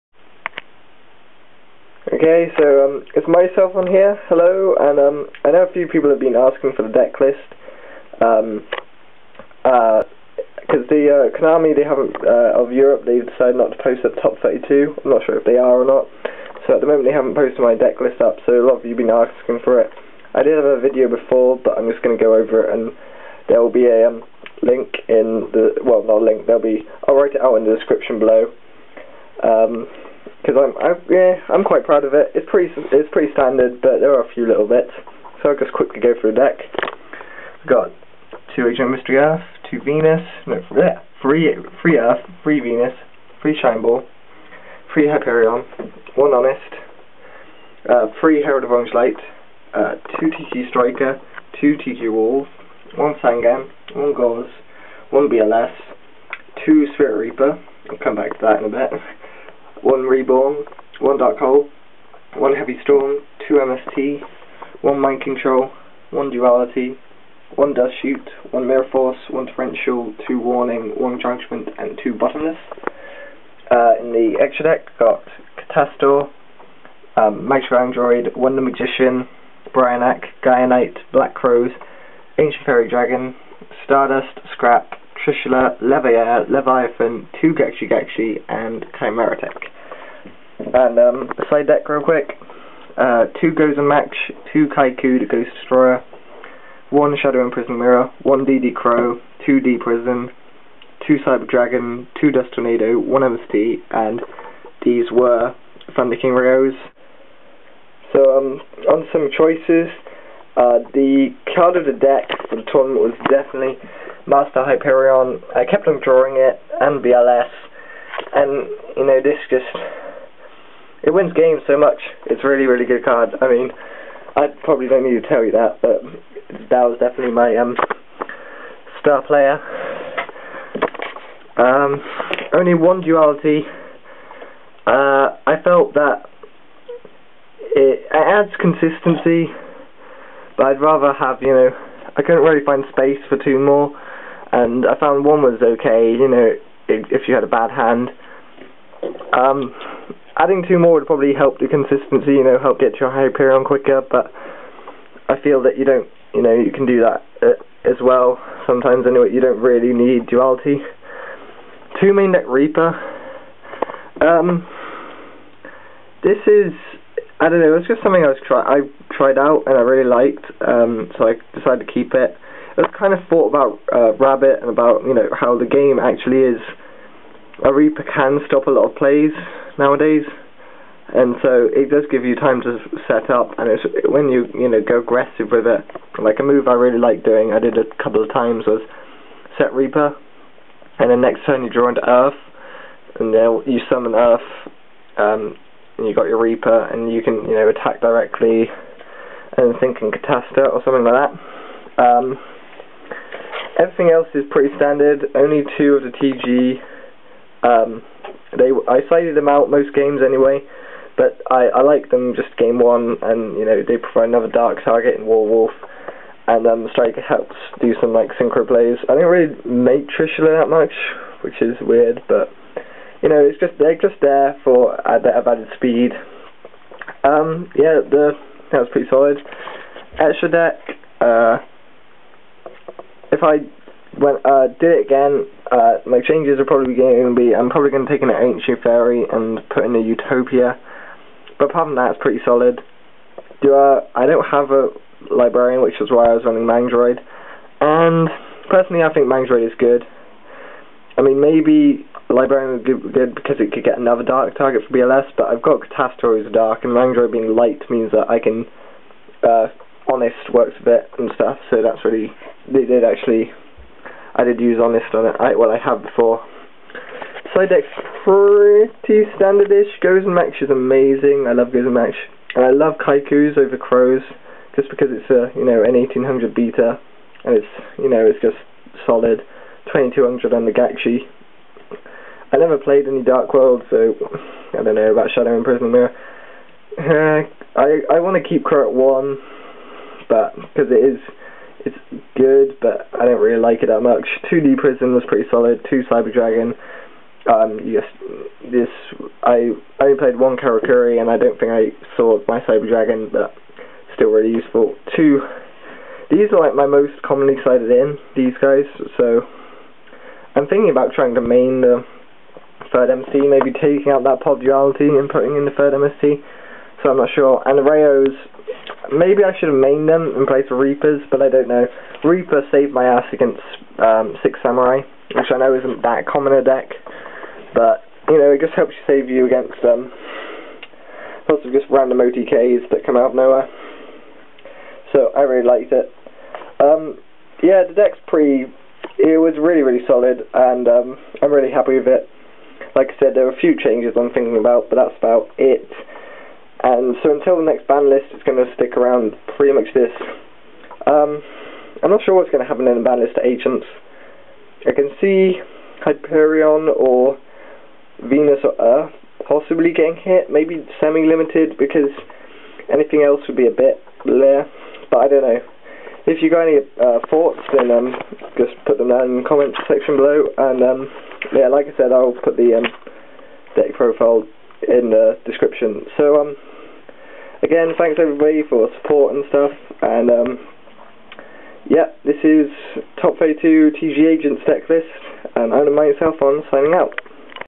Top 32 YCS Brighton Deck Profile - T.G. Agents
Decklist is below.

Monsters (25)
3x The Agent of Creation - Venus
3x The Agent of Mystery - Earth
3x Master Hyperion
3x Herald of Orange Light
3x Mystical Shine Ball
1x Honest
1x Sangan
2x T.G. Warwolf
2x T.G. Striker
1x Black Luster Soldier, Envoy of the Beginning
1x Gorz, the Emissary of Darkness
2x Spirit Reaper

Spells (7)
1x Dark Hole
1x Monster Reborn
1x Heavy Storm
2x Mystical Space Typhoon
1x Mind Control
1x Pot of Duality

Traps (8)
1x Trap Dustshoot
1x Torrential Tribute
2x Solemn Warning
1x Solemn Judgment
2x Bottomless Trap Hole
1x Mirror Force

1x T.G. Wonder Magician
1x Ally of Justice Catastor
1x Magical Android
1x Brionac, Dragon of the Ice Barrier
1x Gaia Knight the Force of Earth
1x Black Rose Dragon
1x Scrap Archfiend
1x Stardust Dragon
1x Scrap Dragon
1x Trishula, Dragon of the Ice Barrier
2x Gachi Gachi Gantetsu
1x Leviair the Sea Dragon
1x Leviathan Dragon
1x Chimeratech Fortress Dragon

Side Deck (15)
2x Cyber Dragon
2x Kycoo the Ghost Destroyer
1x D.D. Crow
2x Gozen Match
2x Dimensional Prison
1x Shadow-Imprisoning Mirror
2x Thunder King Rai-Oh
2x Dust Tornado
1x Mystical Space Typhoon

Really happy with how the deck ran. I've been toying around with the deck for a while and this is the most solid I've managed to make it, so really pleased. No Archlord Kristyas, because I feel they're not necessary and too easy to stop. I'd rather have Reaper which i could play early game, mid game and late game.